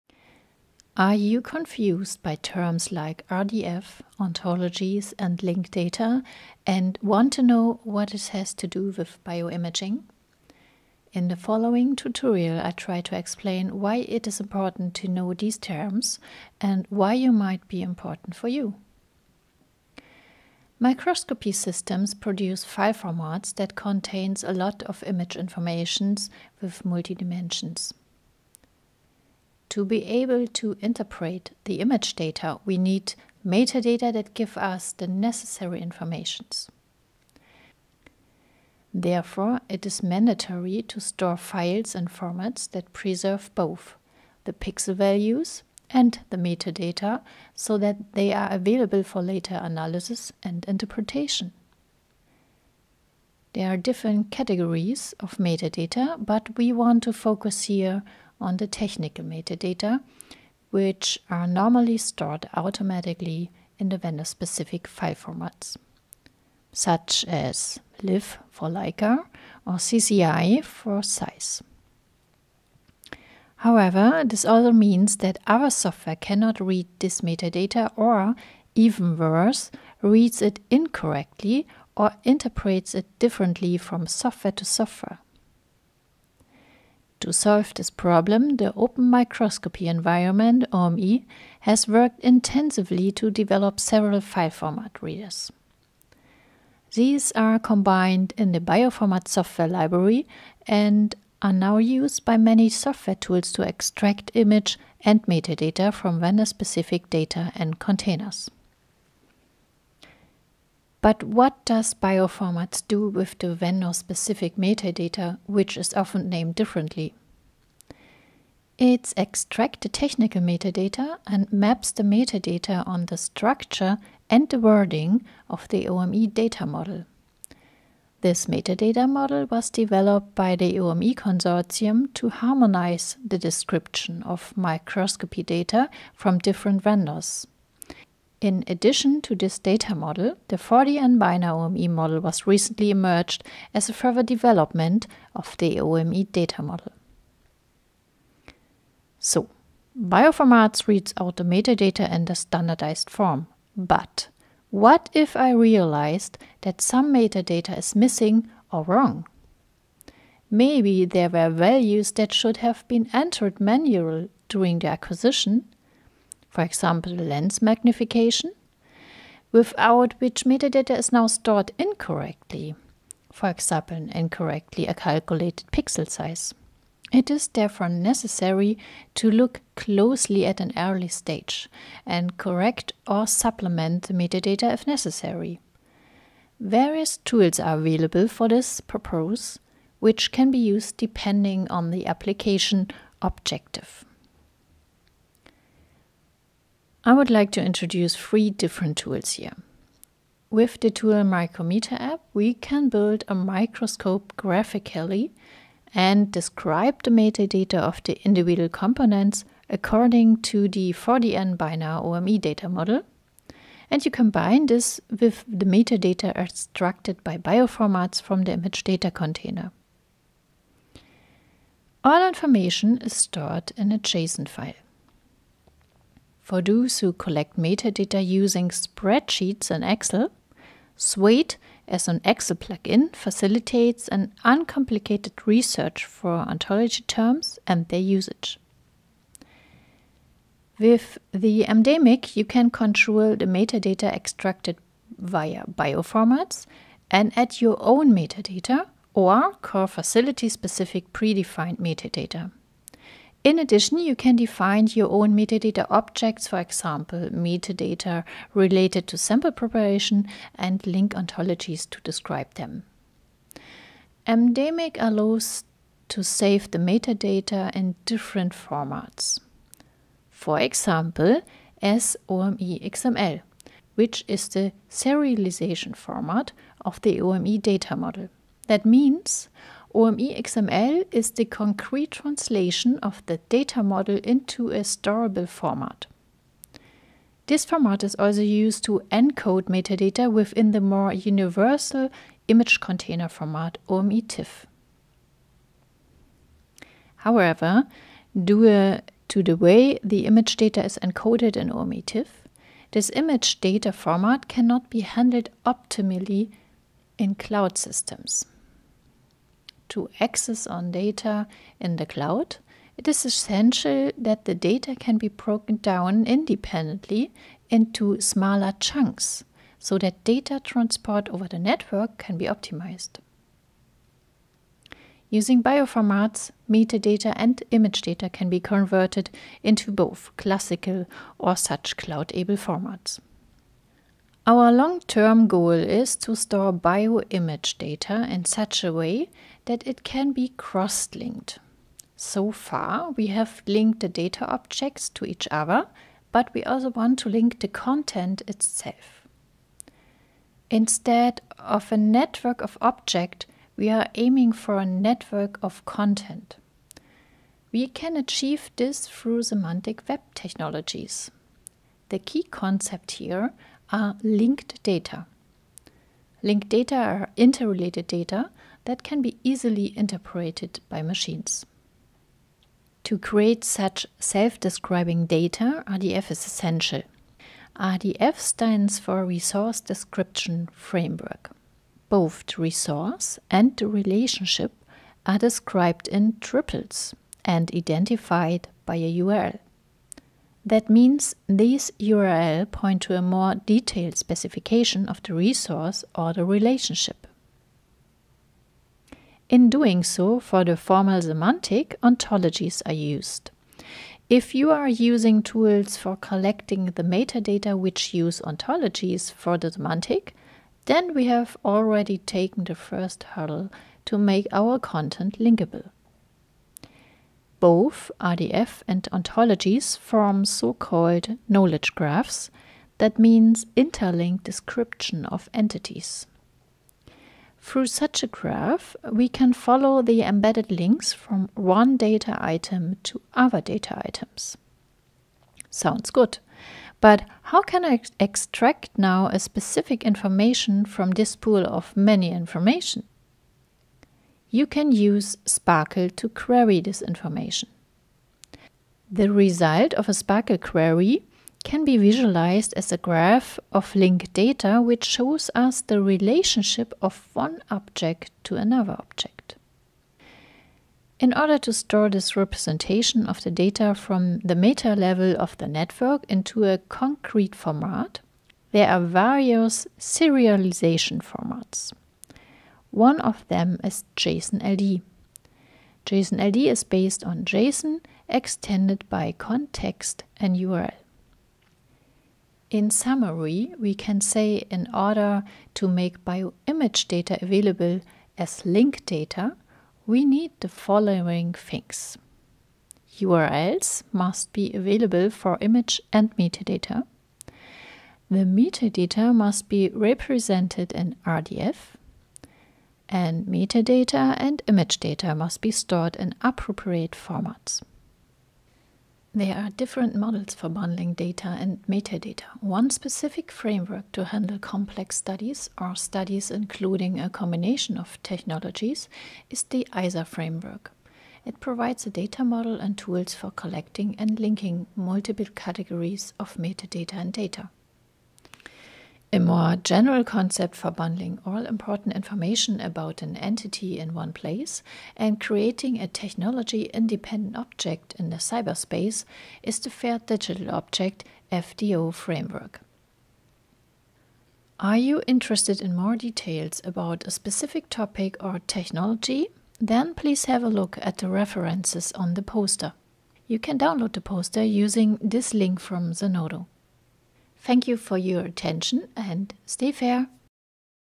Structuring of Data and Metadata in Bioimaging
Provides an overview of contexts, frameworks, and models from the world of bioimage data as well as metadata. Visualizes the techniques for structuring this data as Linked Data.

- Types of metadata
- Data formats
- Data Models Microscopy Data
- Tools to edit/gather metadata
- ISA Framework
- FDO Framework
- Ontology
- RDF
- JSON-LD
- SPARQL
- Knowledge Graph
- Linked Data
- Smart Data

Cite as:
Kunis, S. (2022, August 24). Structuring of Data and Metadata in Bioimaging: Concepts and technical Solutions in the Context of Linked Data. Zenodo.

This video is based on the scientific poster:
Kunis, S., & Dohle, J. (2022). Structuring of Data and Metadata in Bioimaging: Concepts and technical Solutions in the Context of Linked Data. Zenodo.

Funded by the Deutsche Forschungsgemeinschaft (DFG, German Research Foundation) - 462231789